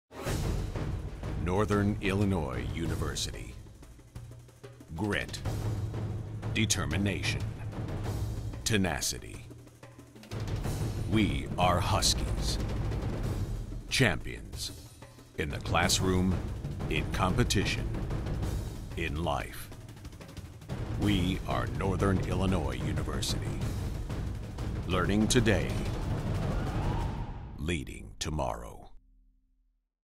NIU Orange Bowl institutional spot
Northern Illinois University institutional message developed for bowl game telecasts, including 2013 Orange Bowl game featuring NIU vs. Florida State on ESPN.